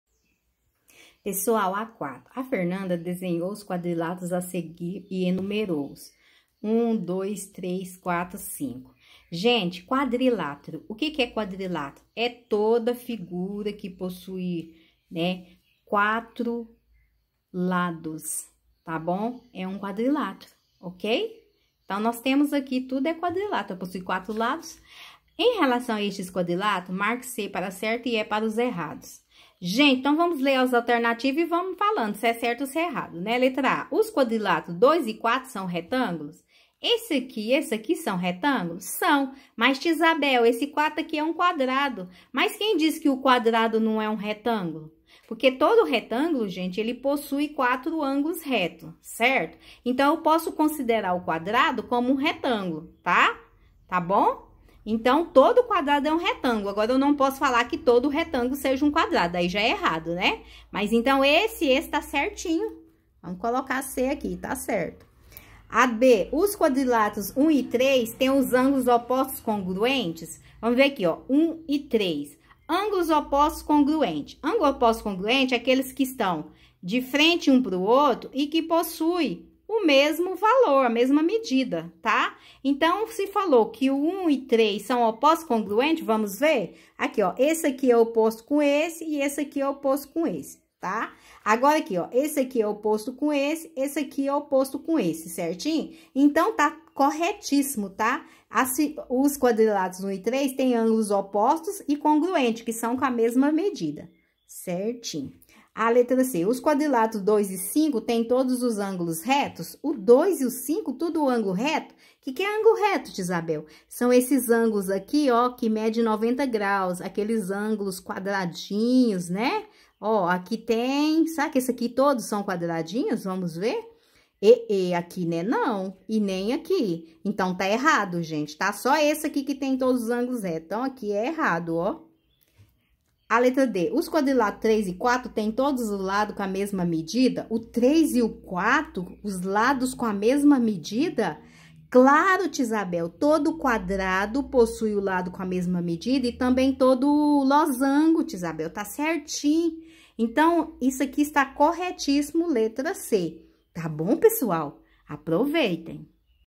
Fernanda desenhou os quadriláteros a seguir e enumerou-os.
Nesse vídeo vamos comparar retângulos, ângulos  e medidas de lados de quadriláteros.